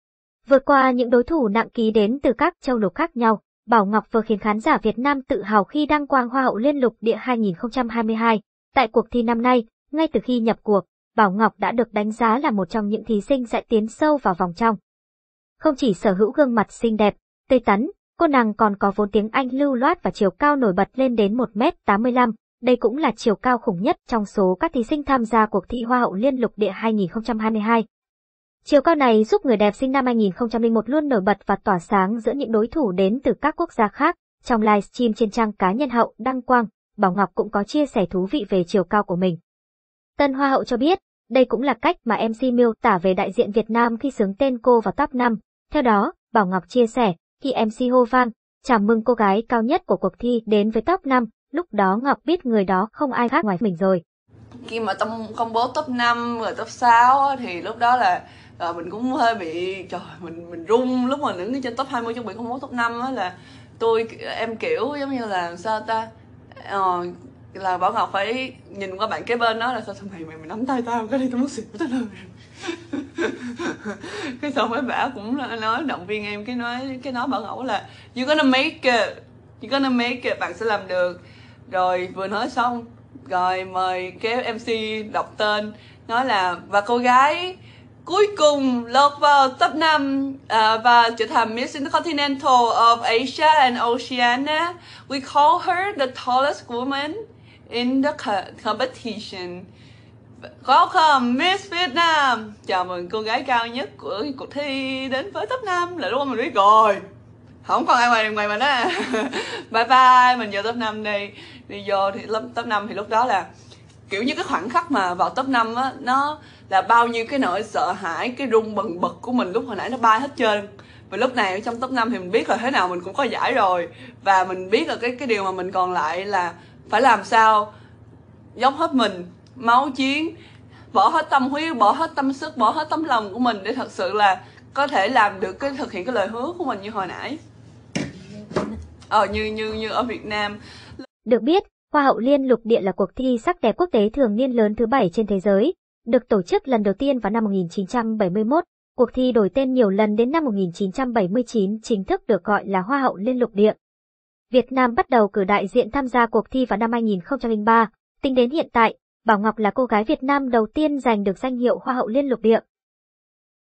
Bảo Ngọc áp đảo dàn thí sinh quốc tế tại Hoa hậu liên lục địa 2022 với chiều cao ‘khủng’
✥ Tại cuộc thi Hoa hậu Liên lục địa 2022, ngay từ khi nhập cuộc, Bảo Ngọc đã được đánh giá là một trong những thí sinh sẽ tiến sâu vào vòng trong.

Không chỉ sở hữu gương mặt xinh đẹp, tươi tắn, cô nàng còn có vốn tiếng Anh lưu loát và chiều cao nổi bật lên đến 1m85.Đây cũng là chiều cao khủng nhất trong số các thí sinh tham gia cuộc thi Hoa hậu Liên lục địa 2022.

Chiều cao này giúp người đẹp sinh năm 2001 luôn nổi bật và tỏa sáng giữa những đối thủ đến từ các quốc gia khác.